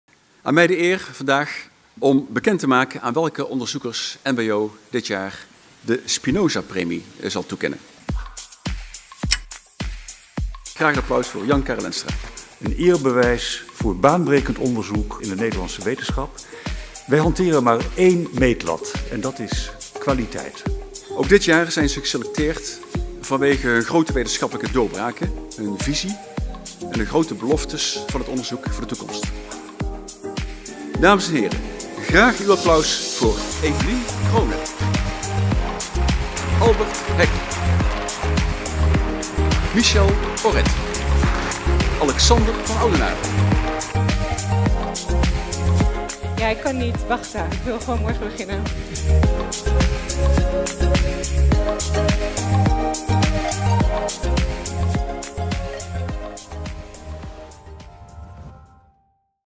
Bekendmaking NWO-Spinozalaureaten 2017
Op 16 juni maakte NWO de winnaars van de NWO-Spinozapremie 2017 bekend: ontwikkelingspsychologe Eveline Crone, analytisch chemicus Albert Heck, fysisch chemicus Michel Orrit en bioloog en biofysicus Alexander van Oudenaarden.
De bekendmaking vond plaats tijdens het evenement Bessensap (bsap17).